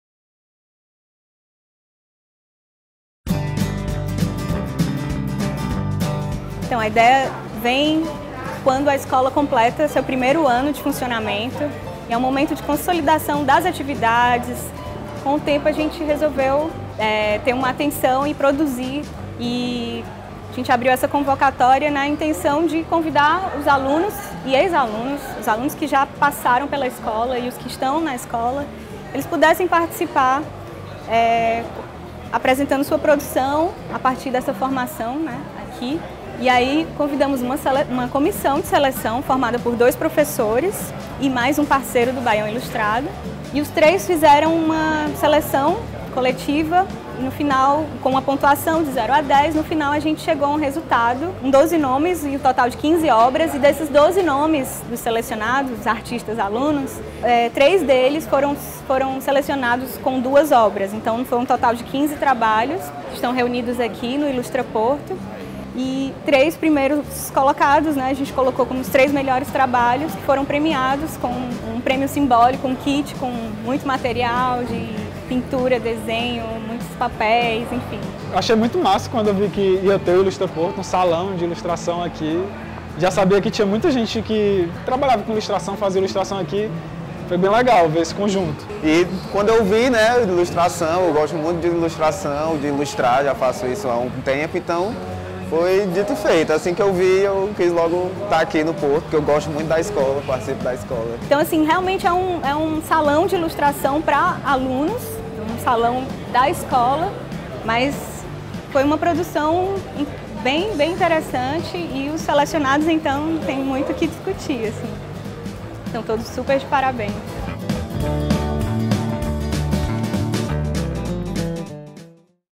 Abertura do Ilustra Porto – 1° Salão de Ilustração da Escola Porto Iracema das Artes
O Ilustra Porto reúne a produção de alunos e ex-alunos nos 15 meses de atividades da Escola Porto Iracema das Artes. A exposição é o resultado da produção dos Cursos Básicos em Artes Visuais e Multimídias como o de Artes Gráficas, Fotografia Digital, Arte Urbana e Animação de Jogos Eletrônicos. Na abertura da exposição, os selecionados receberam  certificados, além da premiação para os três melhores trabalhos, que incluem as obras de Daniel Chastinet (1º lugar), Davi Costa Alves (2º lugar) e Erica Rodrigues (3º lugar).

O Ilustra Porto segue em cartaz no pátio da Escola até o dia 20 de dezembro.